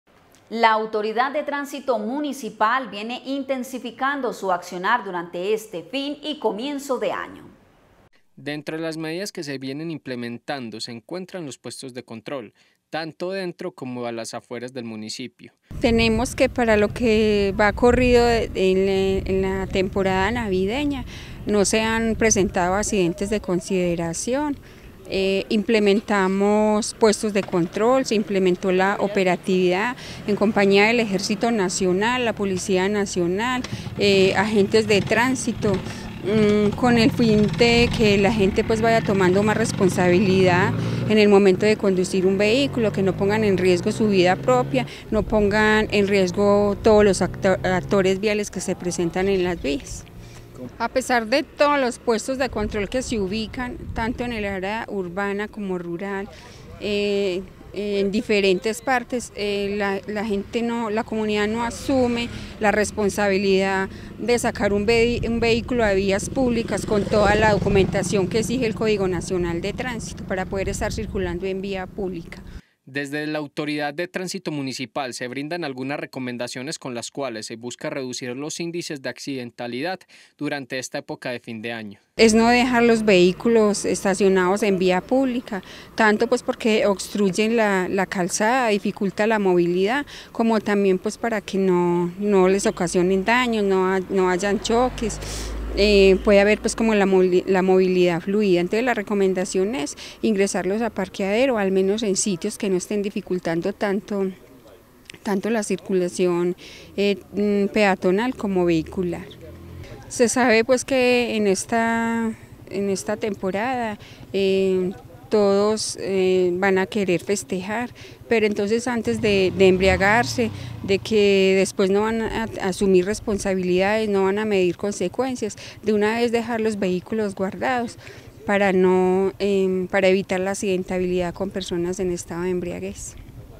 TRÁNSITO INTENSIFICA CONTROLES DURANTE EL FIN DE AÑO
La autoridad de tránsito municipal viene intensificando su accionar durante este fin y comienzo de año.